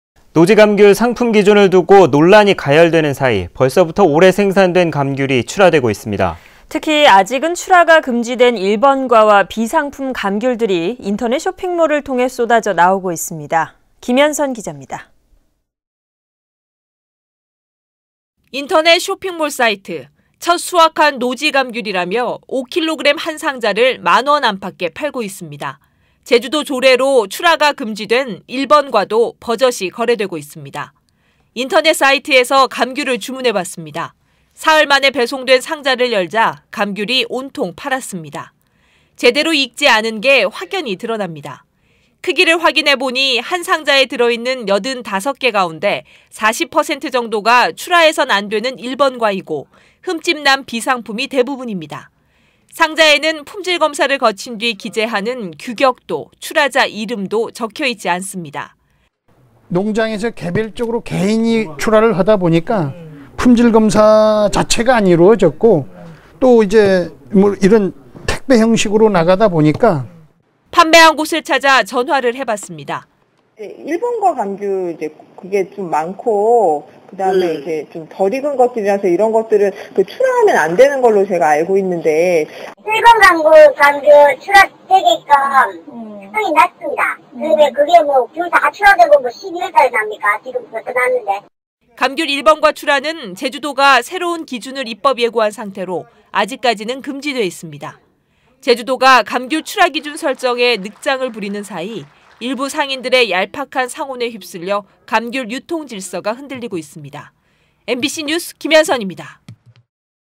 제주MBC 리포트 비상품 감귤인터넷 통해 활개 김연선
◀ＡＮＣ▶
 노지 감귤 상품 기준을 두고
논란이 가열되는 사이,
벌써 올해 생산된
감귤이 출하되고 있습니다.

 특히 출하가 금지된 1번과와
비상품 감귤들이
인터넷 쇼핑몰을 통해 쏟아져 나오면서
감귤 이미지를 흐리지 않을까
우려가 커지고 있습니다.

 김연선 기잡니다.
           ◀ＥＮＤ▶
           ◀ＶＣＲ▶
 인터넷 쇼핑몰 사이트,

 첫 수확한 노지감귤이라며
5kg 한 상자를 만 원 안팎에
팔고 있습니다.

 제주도 조례로 출하가 금지된 1번과도
버젓이 거래되고 있습니다.

 인터넷 사이트에서
감귤을 주문해 봤습니다.

 사흘만에 배송된 상자를 열자,
감귤이 온통 파랗습니다.

 제대로 익지 않은게 확연히 드러납니다.

 크기를 확인해보니,
한 상자에 들어있는 85개 가운데
40% 정도가 출하해선 안되는 1번과이고
흠집 난 비상품이 대부분입니다.

 상자에는 품질검사를 거친 뒤
기재하는 규격도, 출하자 이름도
적혀 있지 않습니다.

 ◀ＳＹＮ▶임상필 감귤특작과장 /제주도
"감귤 농가에서 직접 출하하니 품질검사도 거치지 않았고.. 특히 택배로 되다 보니"

 판매한 곳을 찾아 전화를 해봤습니다.

 ◀ＳＹＮ▶00농원 감귤 판매처(전화녹취)
(1번과나 상처난 감귤들은 판매하지 않도록 알고 있는데?)
"1번 감귤 출하되게끔 허용 났습니다.. 귤 다
출하되고 12월에 납니까.. 벌써 (허용)됐는데"

 감귤 1번과 출하는
제주도가 새로운 기준을 입법예고한 상태로
아직까지는 금지돼 있습니다.

 제주도가
감귤 출하기준 설정에 늦장을 부리는 사이,
일부 상인들의 얄팍한 상혼에 휩쓸려
감귤 유통질서가 흔들리고 있습니다.

 MBC뉴스 김연선입니다.